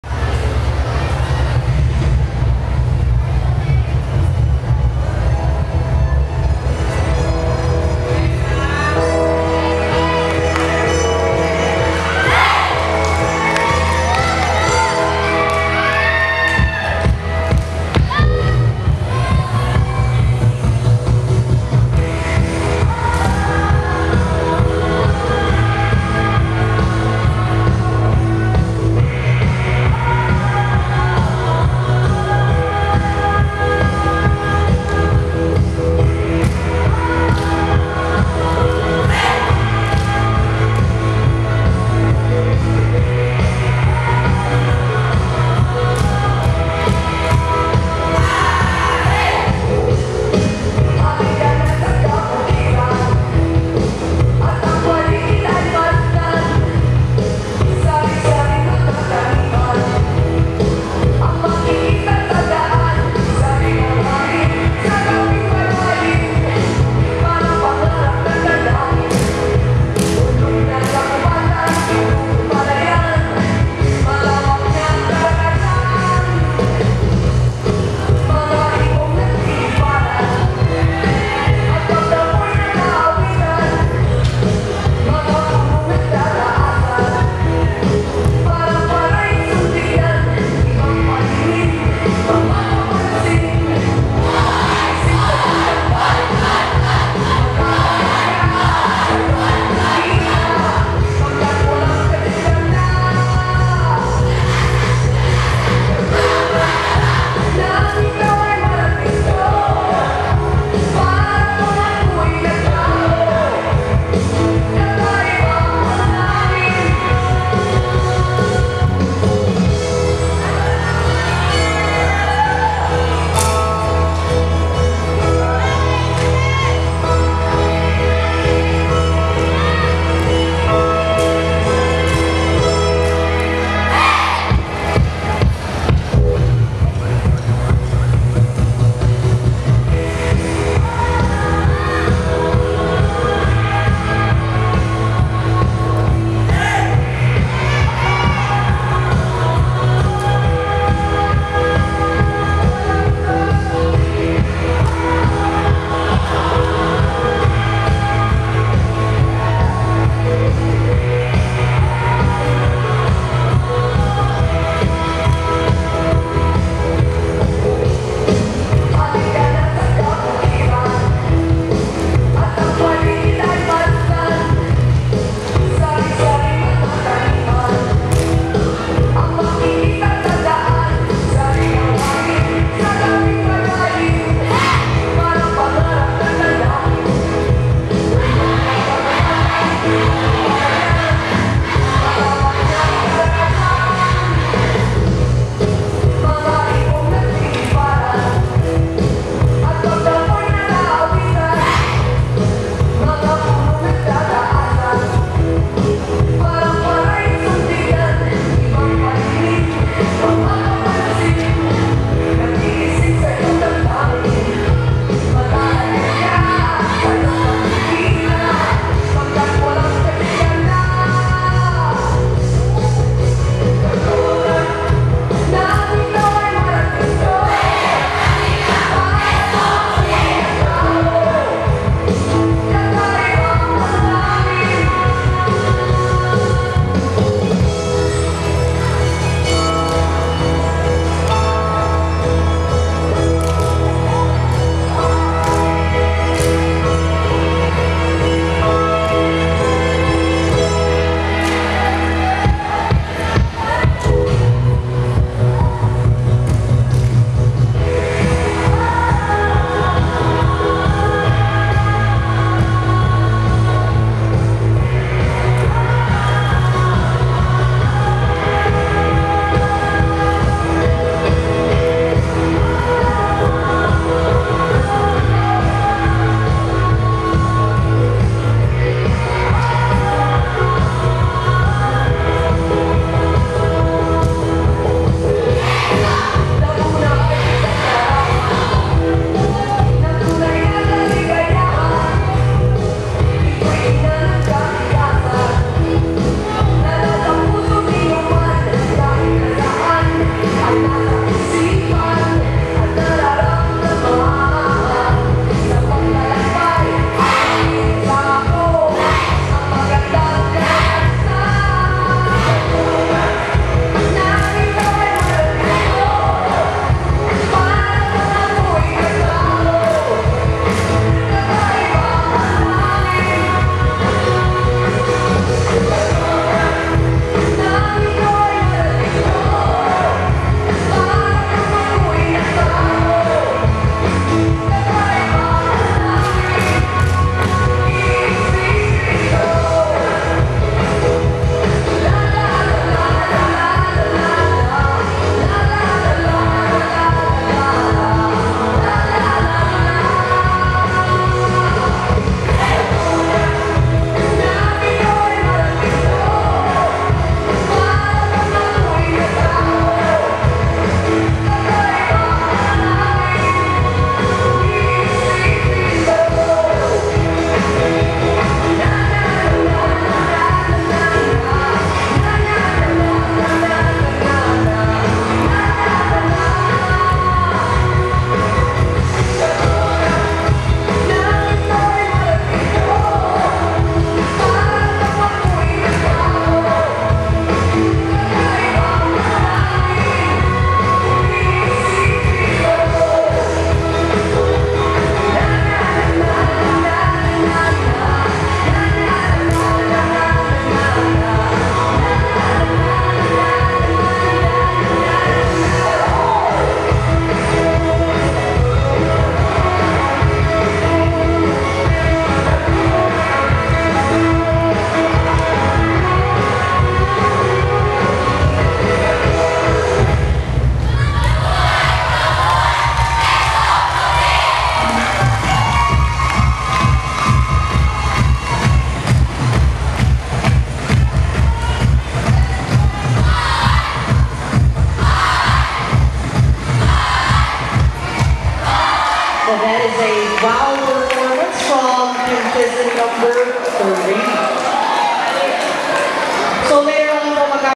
Second Place 2016 Laguna Musikahan - Street Dance Competition
by: Laguna Warriors
Dec. 1, 2016
San Pedro Central Elem. Sch.
San Pedro, Laguna